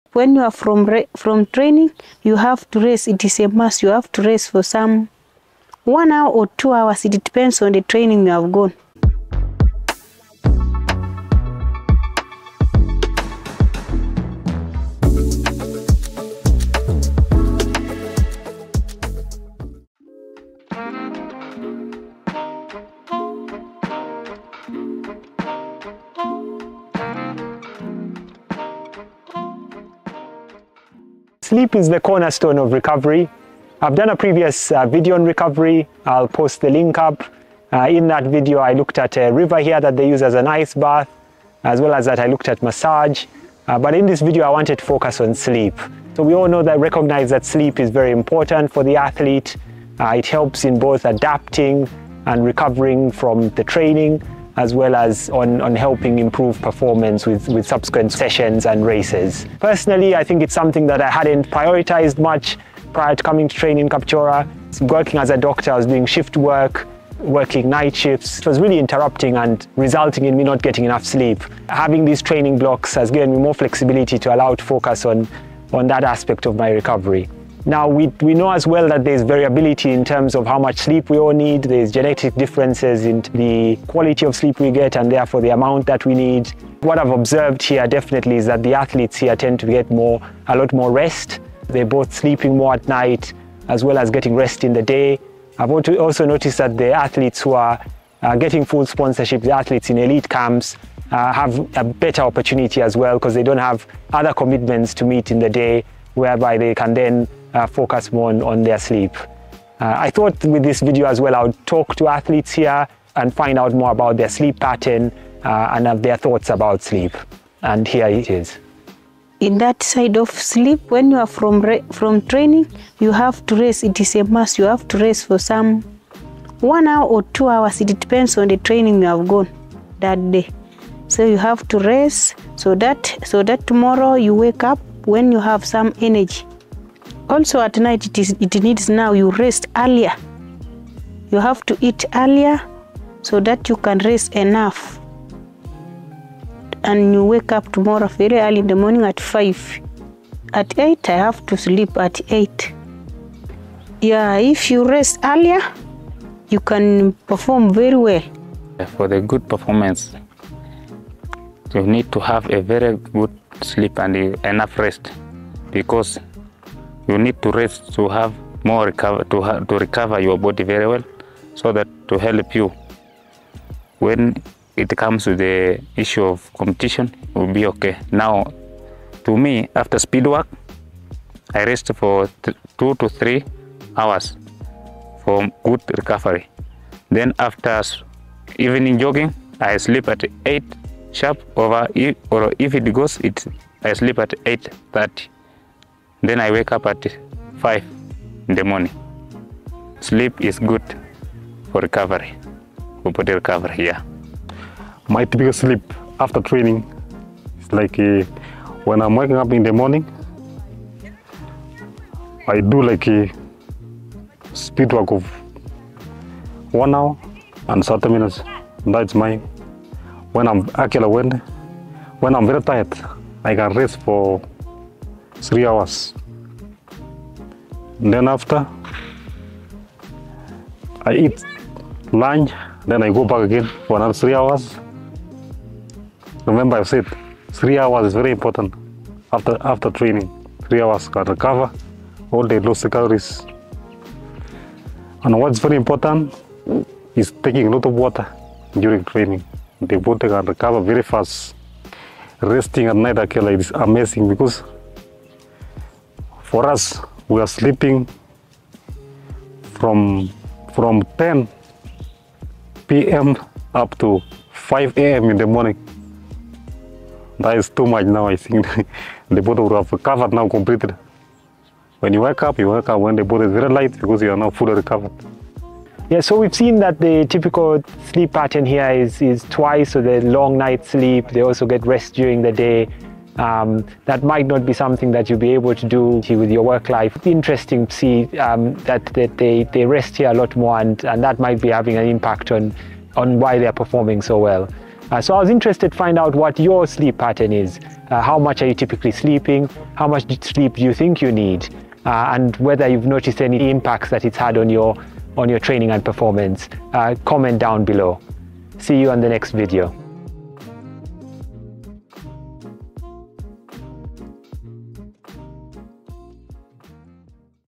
Ugandan athletes talk about their sleep
Sharing more insights from my training in Kapchorwa, Uganda. We all know that sleep is important for training. Athletes here rest and sleep a lot. Athletes in elite camps are privileged to have less commitments and use that opportunity to rest even more. I speak with athletes to discuss their sleep patterns and their thoughts on sleep.

Timestamps
00:31 Introduction
01:59 Mercyline Chelangat
02:38 Elphas Ndiwa
03:24 Isaac Kibet
04:35 Summary

Assisted with filming by Jacob Mutela/Charis Media